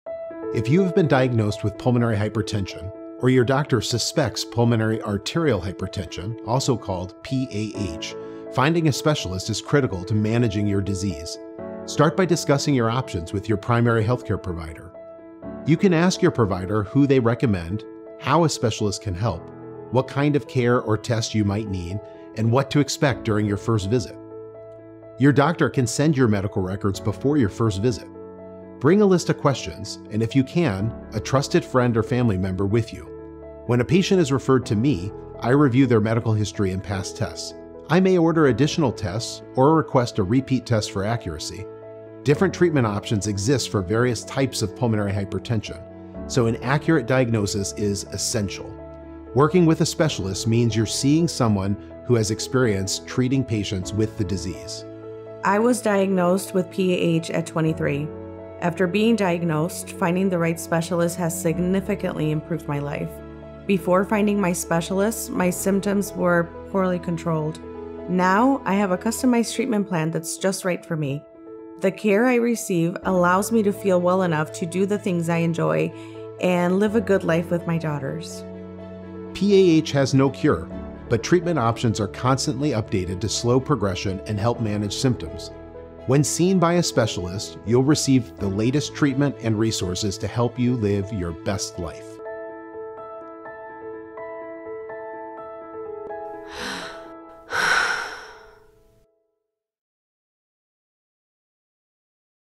Finding a Specialist for Pulmonary Arterial Hypertension (PAH)
Pulmonary arterial hypertension (PAH) is a rare, progressive lung disease caused when the tiny arteries in your lungs become narrowed and thickened. Hear how a specialist will help you get the best possible care.

Support for this educational content is provided by Johnson & Johnson.